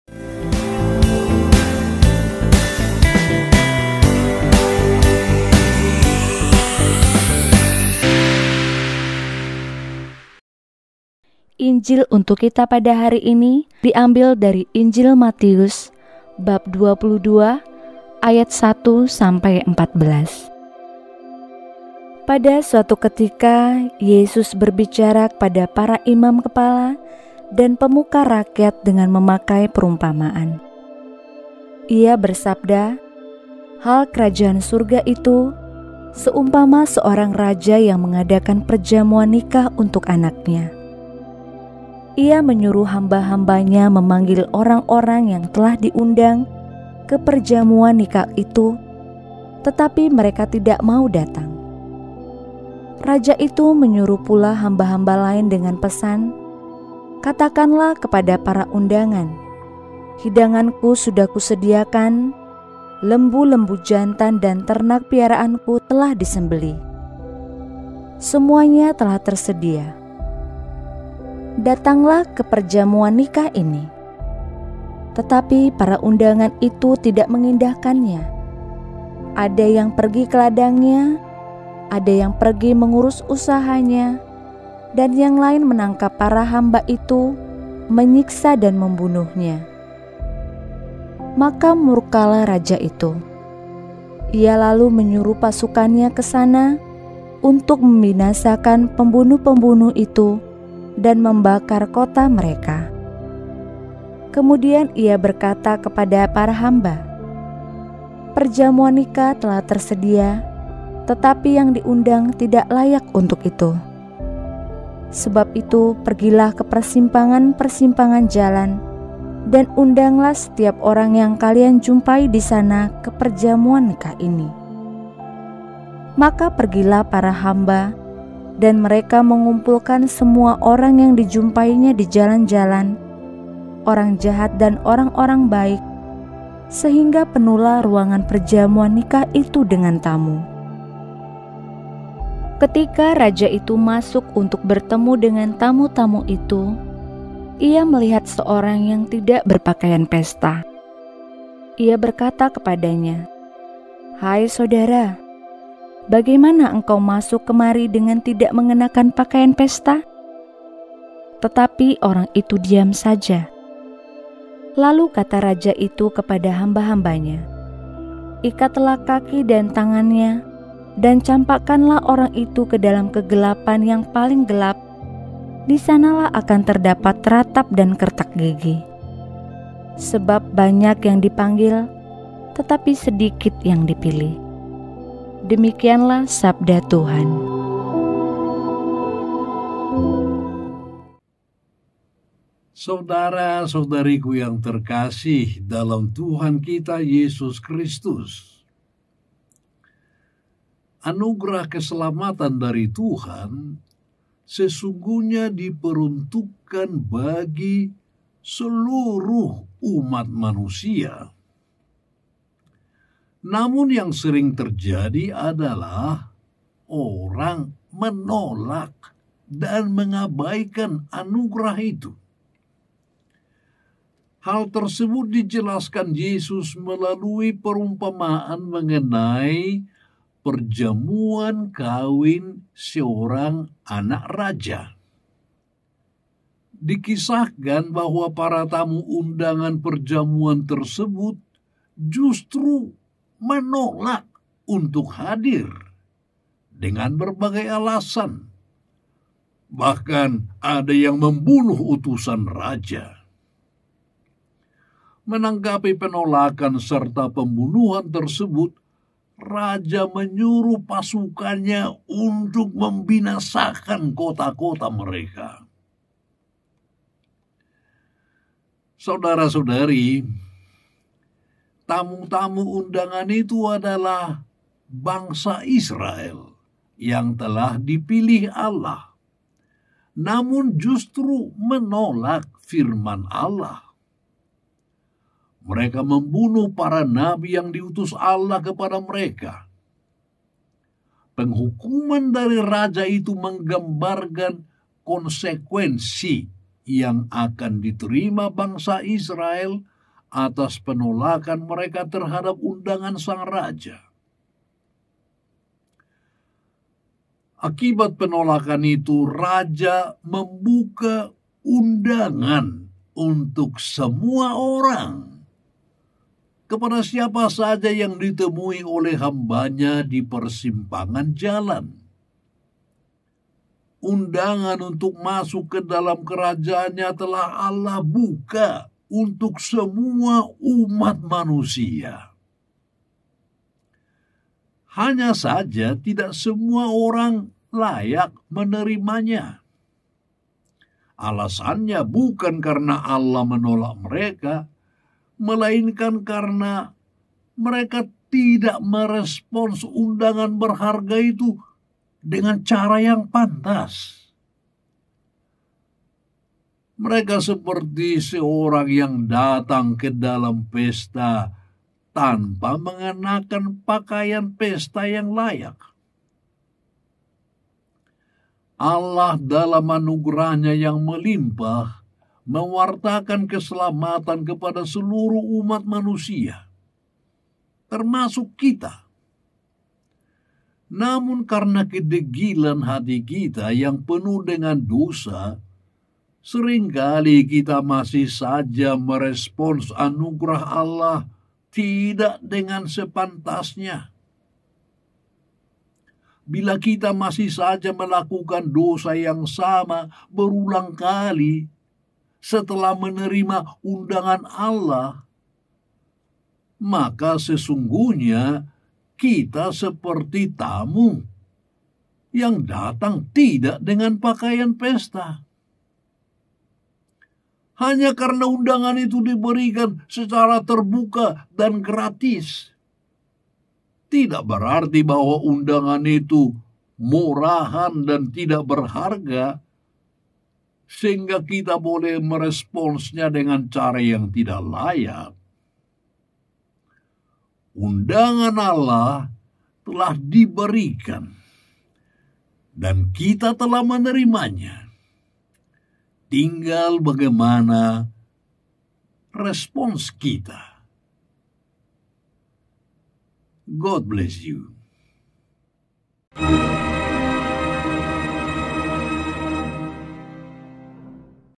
Pekan 20 Kamis Masa Biasa: MERESPONS UNDANGAN BERHARGA DENGAN CARA YANG PANTAS
Undangan untuk masuk ke dalam Kerajaan-Nya telah Allah buka untuk semua umat manusia. Hanya saja, tidak semua orang layak menerimanya. Alasannya bukan karena Allah menolak mereka, melainkan karena mereka tidak merespons undangan berharga itu dengan cara yang pantas. Seperti seorang yang datang ke dalam pesta tanpa mengenakan pakaian pesta yang layak.
Hanya karena undangan itu diberikan secara terbuka dan "gratis" tidak berarti bahwa undangan itu murahan dan tidak berharga sehingga kita boleh meresponsnya dengan cara yang tidak layak. Undangan Allah telah diberikan dan kita telah menerimanya, tinggal bagaimana respons kita.